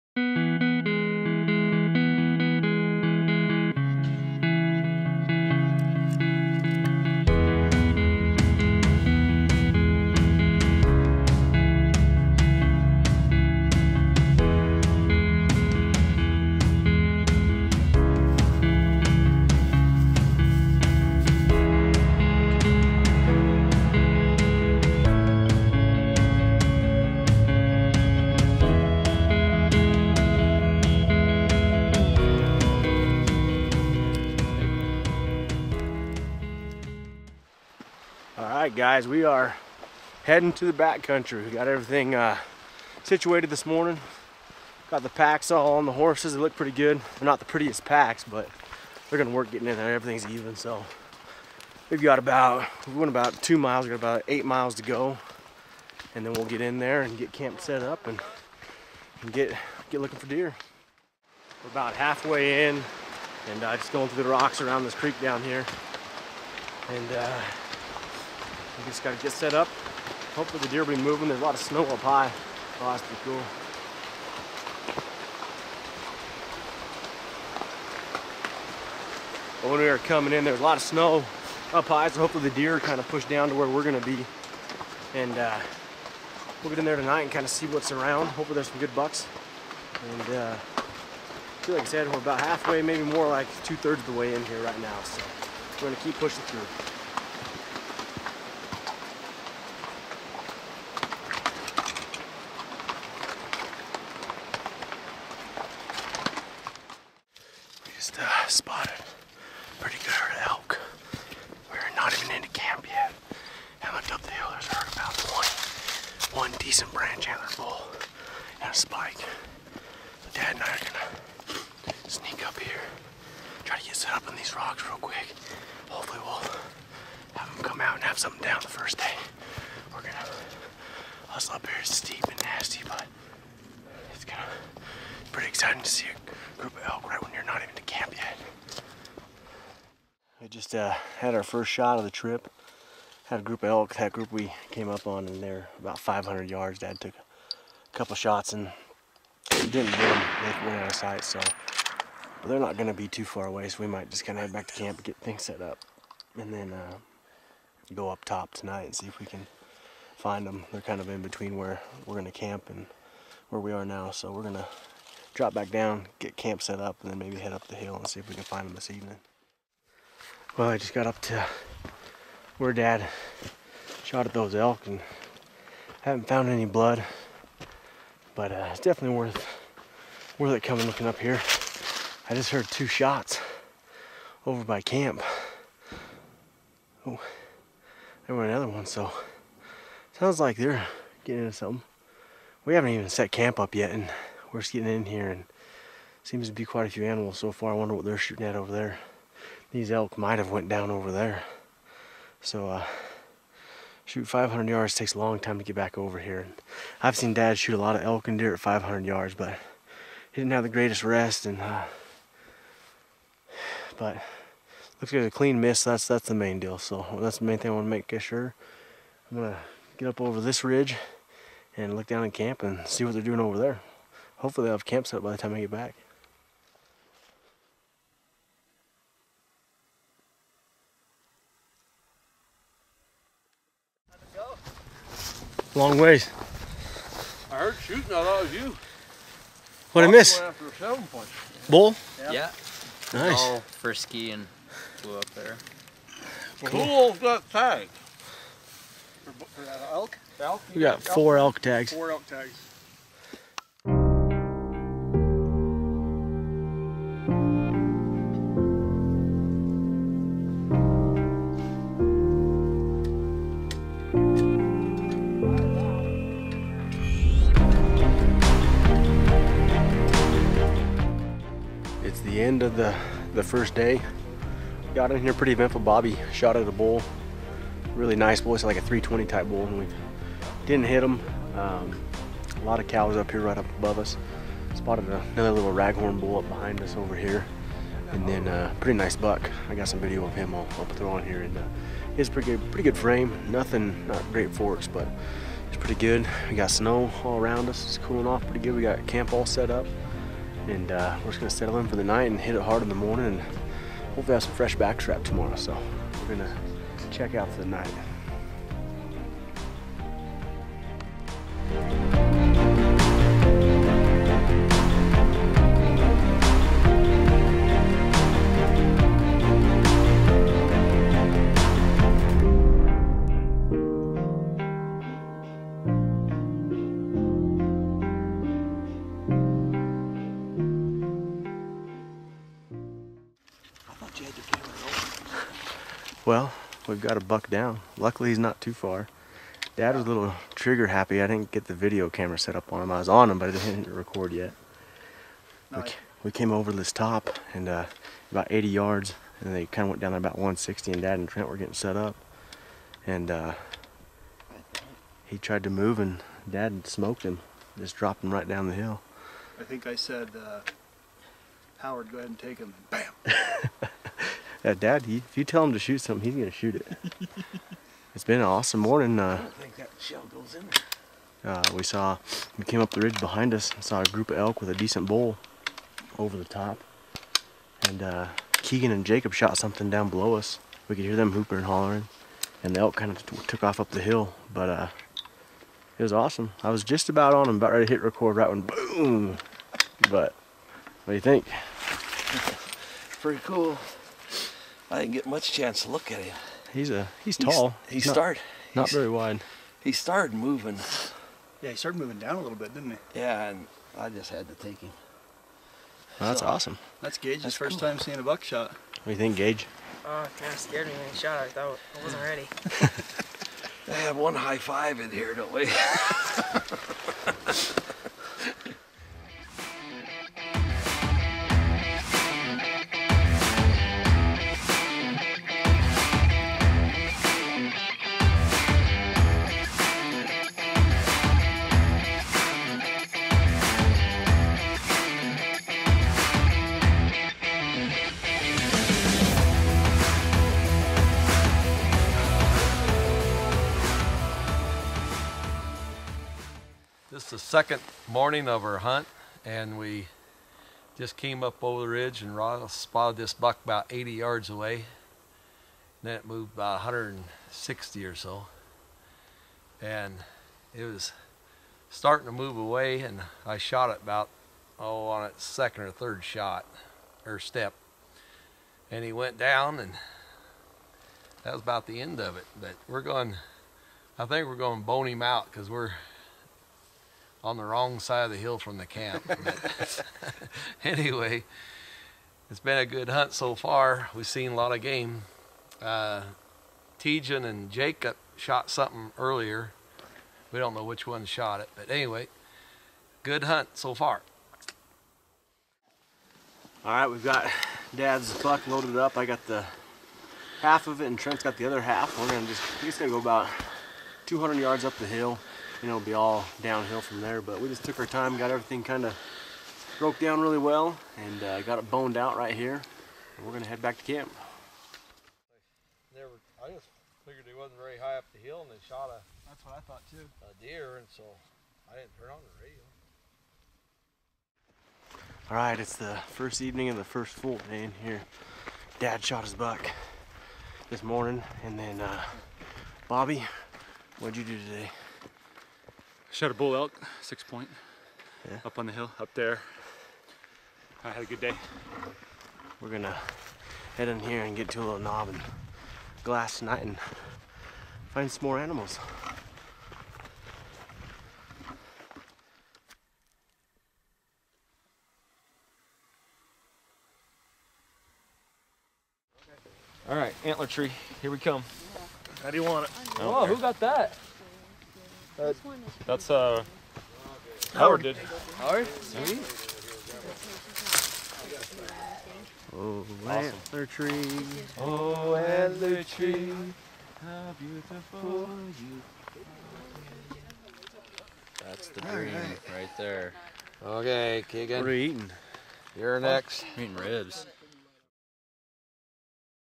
2016 Idaho Backcountry Elk & Deer Hunt - Part 1
In this 3 part series follow Ross around in the backcountry of Idaho chasing elk and deer with his dad and friends!!